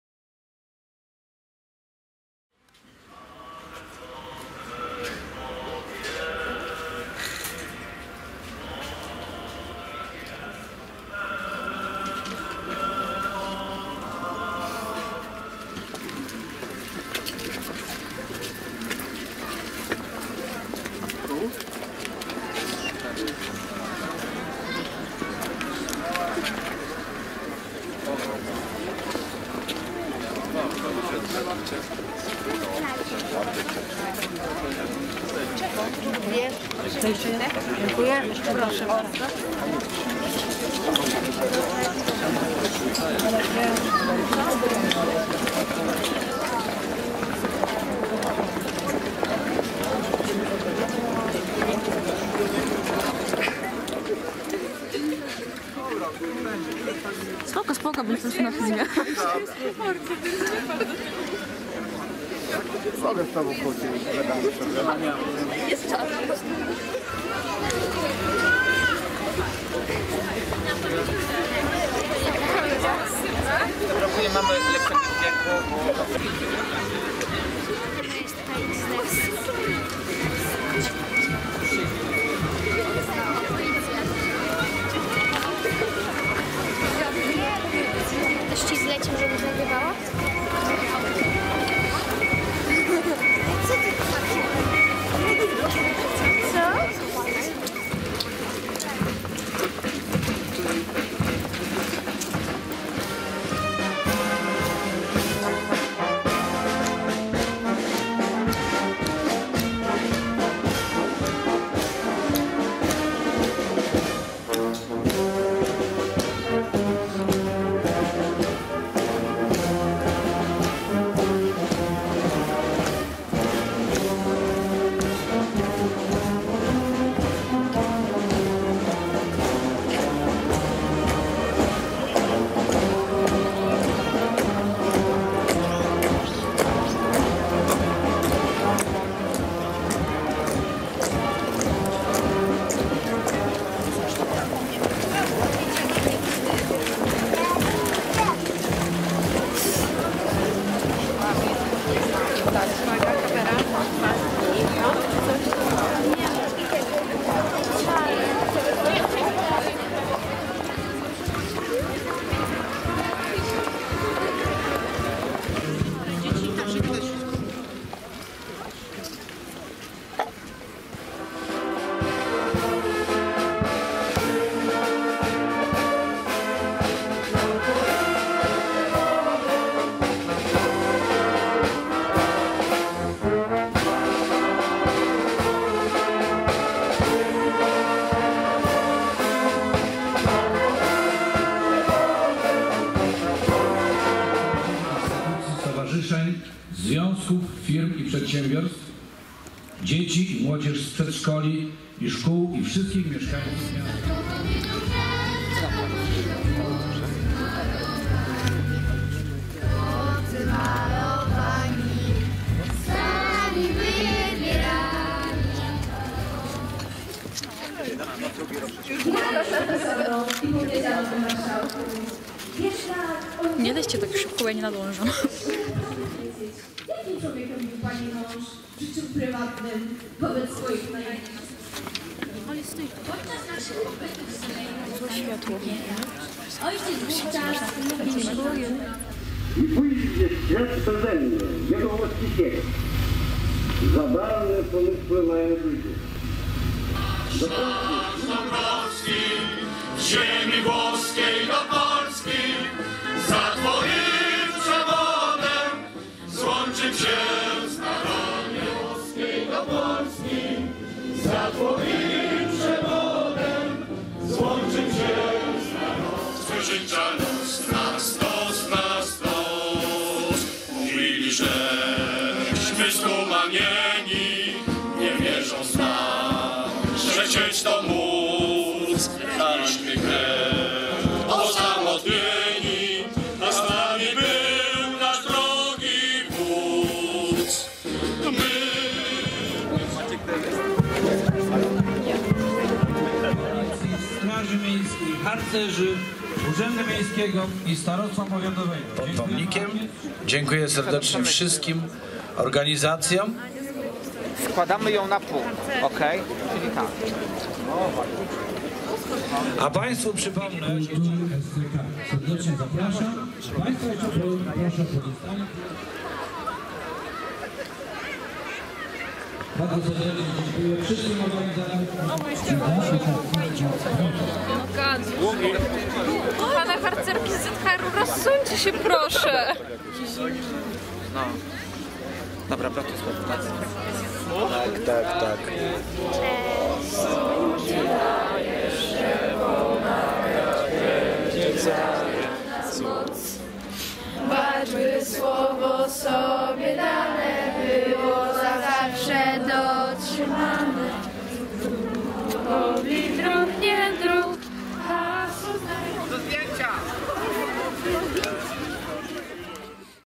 Miejskie obchody Święta Niepodległości. Mielec, 11 listopada 2019.
11 listopada 2019 roku braliśmy udział w Miejskich obchodach Święta Niepodległości. Nasi harcerze uczestniczyli w pochodzie ulicami miasta. Dumnie nieśli biało-czerwoną flagę, rozdawali mniejsze flagi i okolicznościowe gazetki. Pełnili wartę przy pomniku Wolności oraz godnie reprezentowali hufiec.